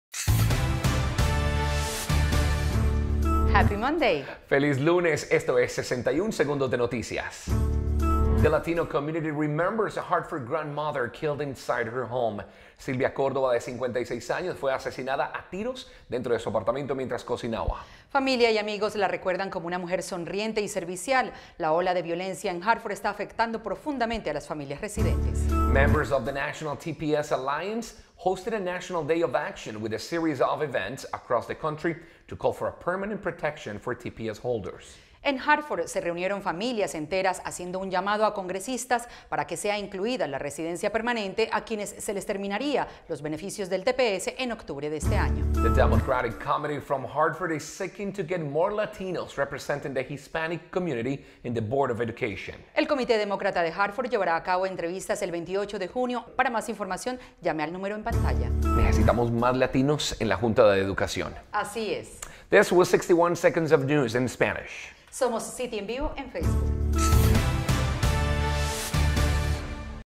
61 Segundo de Noticias: June 14
Community in Hartford still morning the loss of shooting victim; Hartford Democratic Town Committee is seeking more Latino representation in Board of Education